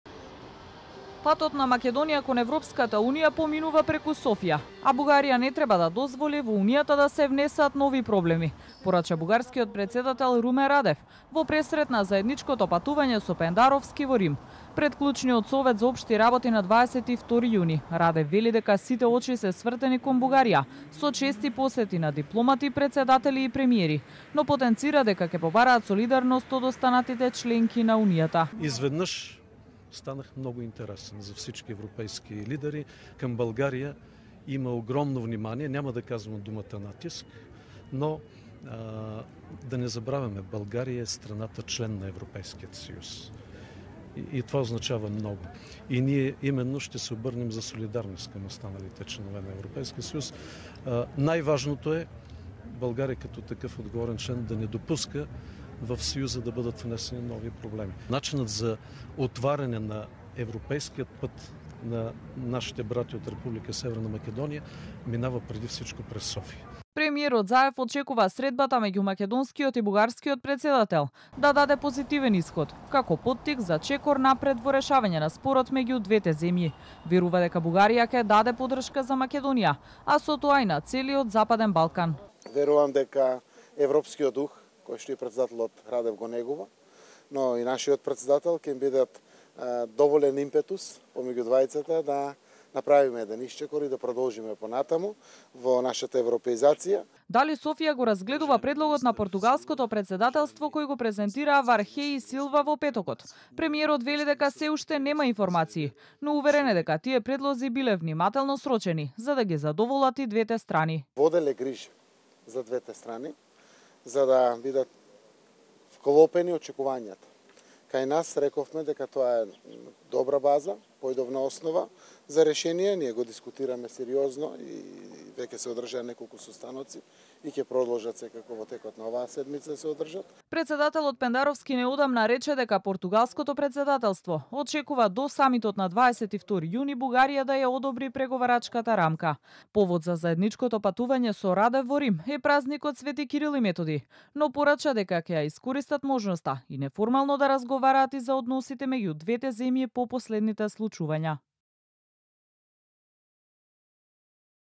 Заедничкото патување на Пендаровски и Радев надеж за напредок во македонско - бугарските односи
Патот на Македонија кон Европската Унија поминува преку Софија, а Бугарија не треба да дозволи таму да се внесат нови проблеми - порача бугарскиот претседател Румен Радев во пресрет заедничкото патување со Пендаровски во Рим.